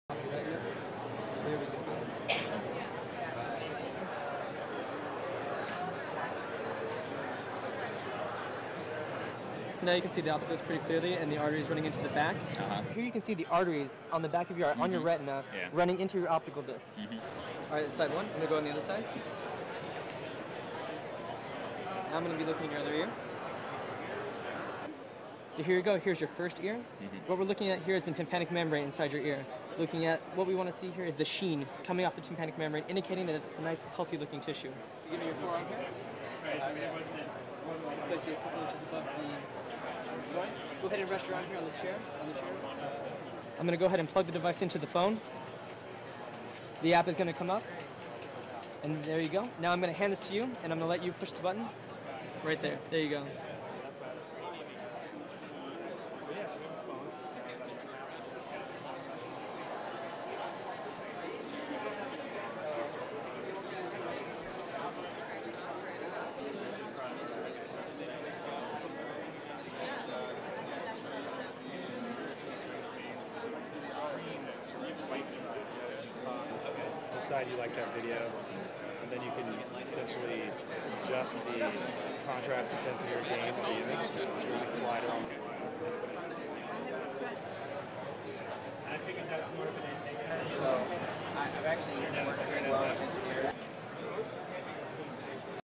Aplicaciones médicas para los smartphones
Medirse la tensión, el ritmo cardíaco o la vista son algunas de las cosas que se pueden hacer con las nuevas aplicaciones médicas para smartphones. Se han presentado bajo el nombre The Smartphone Physical, en Washington.
Tentsioa, ikusmena edo bihotz-murmurioa sakeleko telefonotik neurtzeko aukera hemen da, jada. Washingtonen aurkeztu dira The Smartphone Physical osasun aplikazioak.